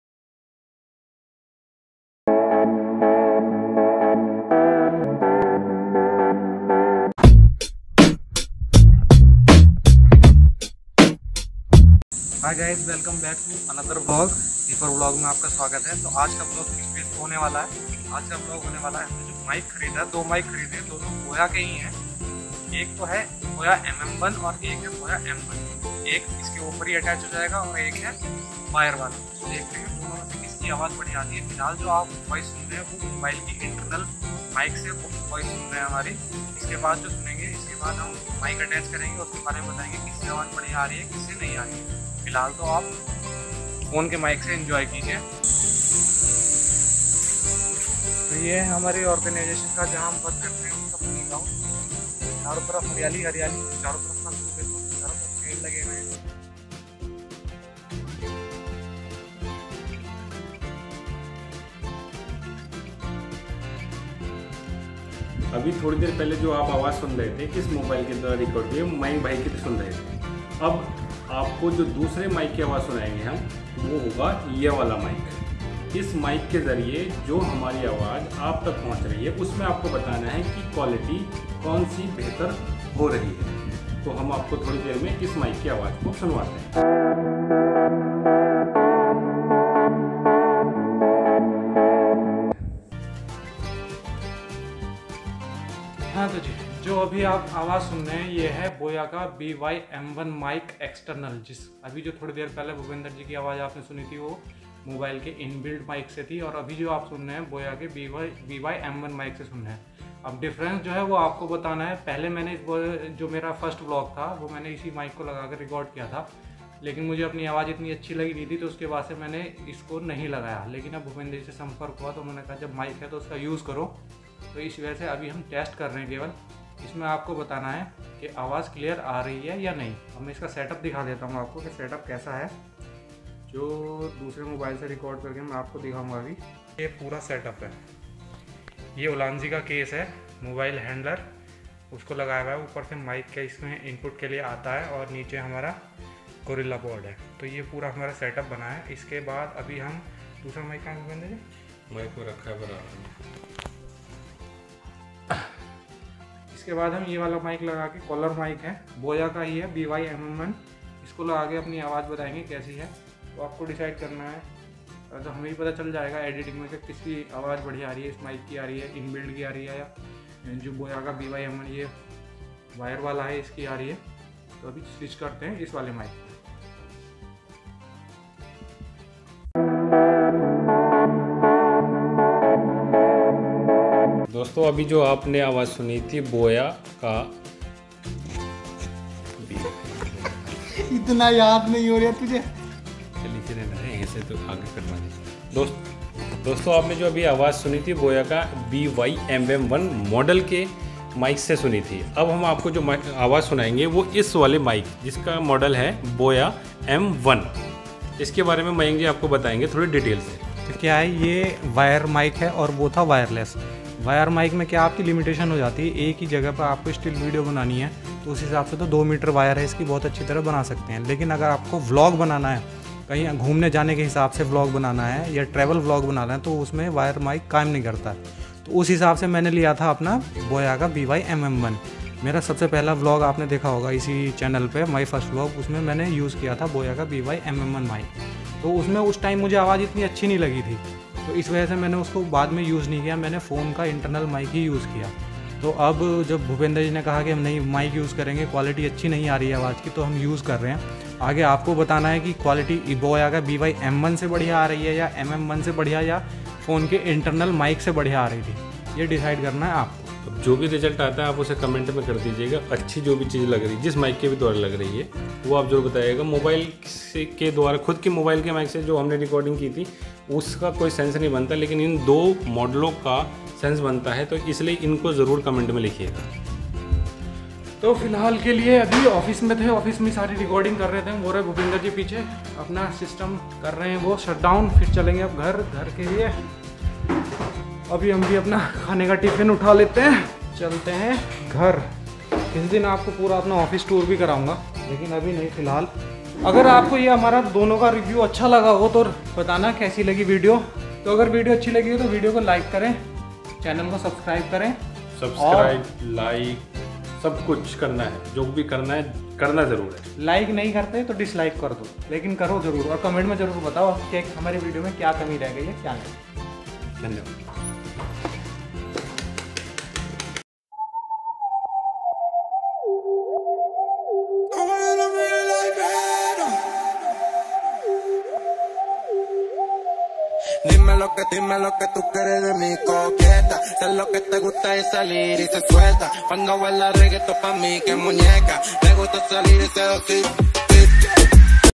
Cheap & best mic for beginner || Boya By MM1 Vs Boya By M1 || Mayank Deep Rajput - MDR
In this video I'll show you the comparision between two mics . Both mics are from Boya company. One is wireless & other is wired. but the voice recording quality of both mic are best for beginners.